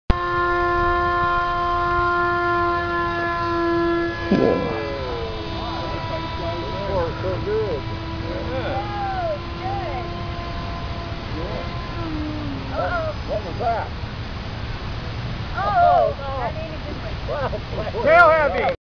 RC Foamy Jet Crash Landing
Here is Jesse test flying a home built jet made by Alec.  The tail is way too heavy.  Nice save by Jesse